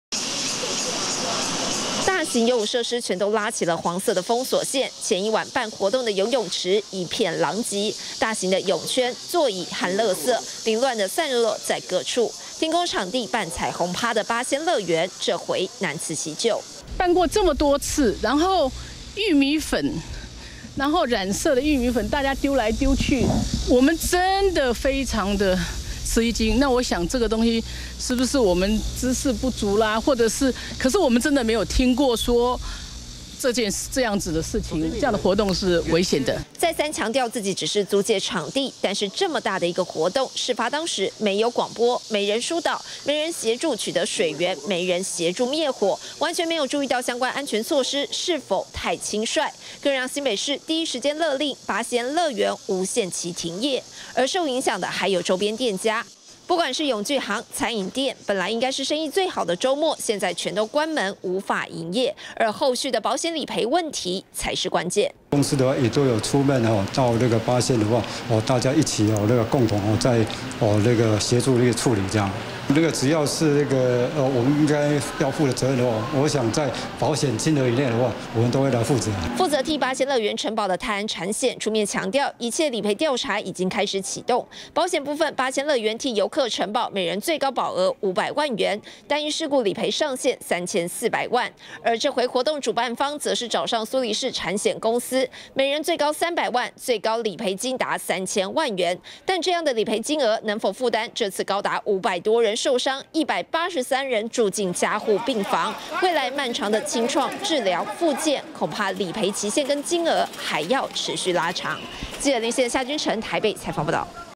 20150628中天新聞 危機應變出問題！ 八仙樂園無限期停業
這起粉塵爆炸意外，截至目前為止造成519人受傷，雖然八仙樂園負責人出面澄清，表示園區只是租借場地，完全沒有參與活動設計，更沒想到「只是玉米粉丟來丟去」，竟然會造成這麼大的危險！但這起意外發生，八仙危機應變能力也引發質疑，新北市府第一時間更是勒令八仙樂園無限期停業，而對受傷家屬來說，後續保險理賠才是問題，八仙樂園跟活動公司雖然各替遊客投保了500萬以及300萬的個人受傷意外險，但後續清創跟復健路漫長，賠償金額究竟夠不夠？能否支付更是關鍵。